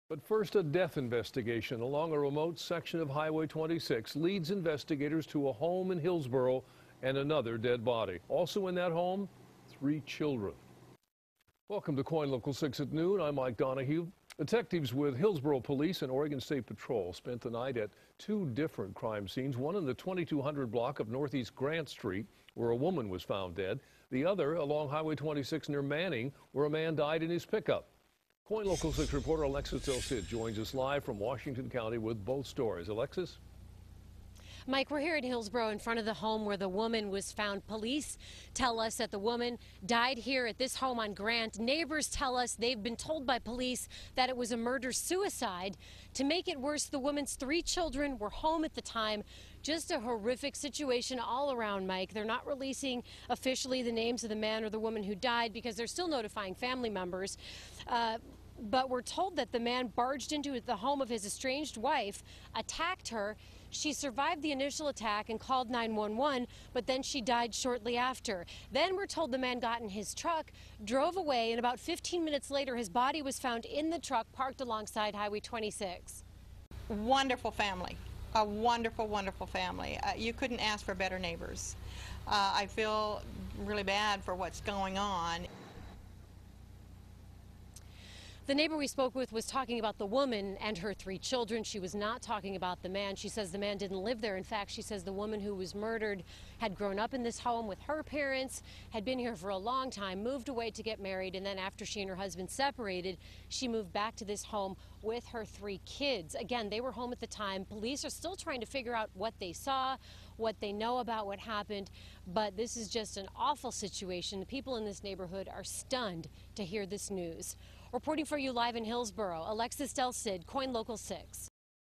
Investigators believe man found dead on Hwy 26 killed wife
Hillsboro police report that a 45-year-old man found dead inside a parked vehicle on Highway 26 in rural Washington County early Monday morning may have killed his estranged wife at her Hillsboro home.